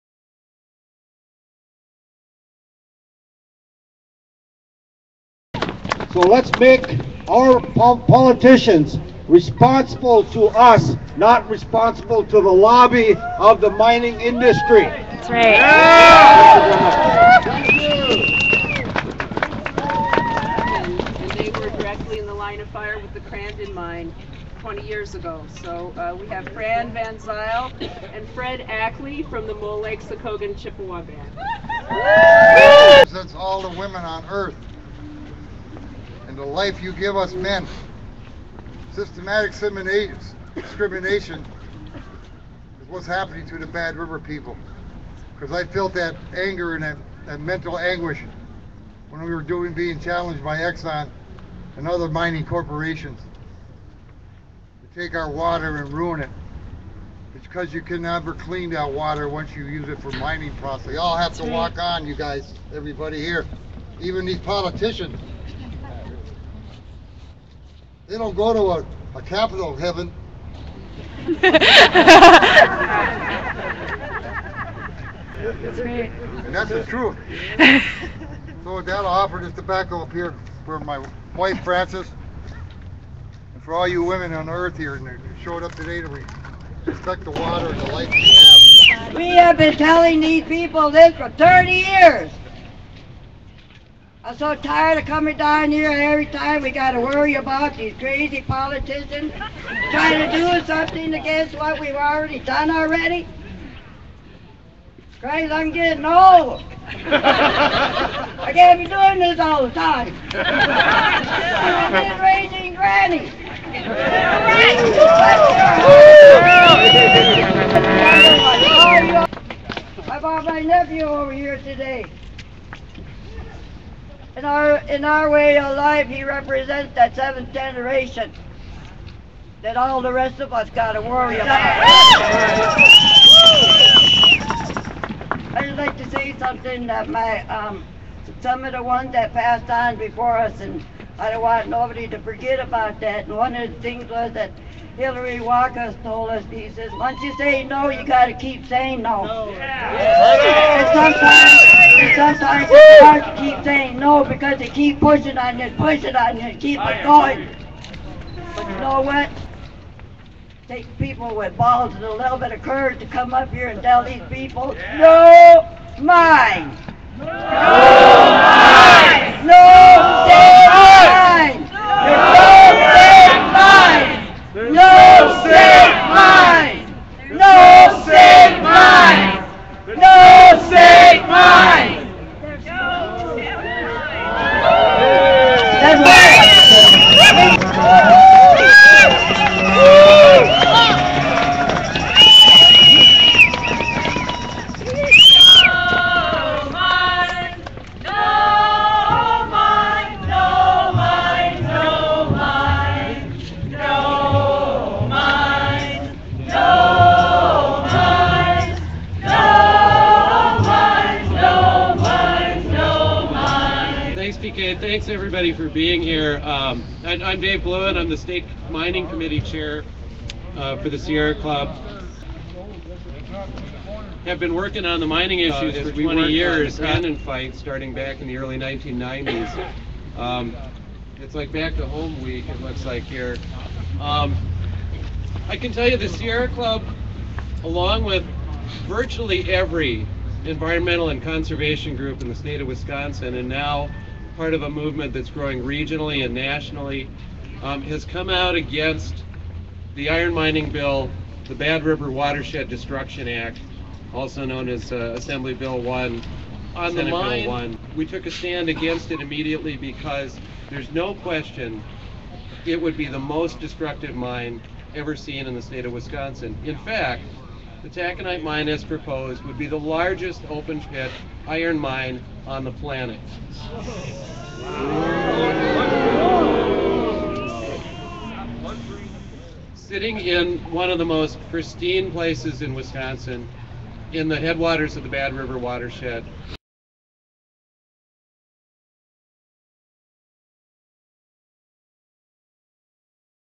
Mole Lake Chippewa Band Speaks a the Capitol
Fran and Fred from Mole Lake Chippewa Band speak at the Capitol.Hundreds gather at the Capitol with organizations from around the state of Wisconsin with members of the Bad River Tribe to tell the state legislature and governor to not throw away Wisconsin's local control and environmental protections for out-of-state mining companies. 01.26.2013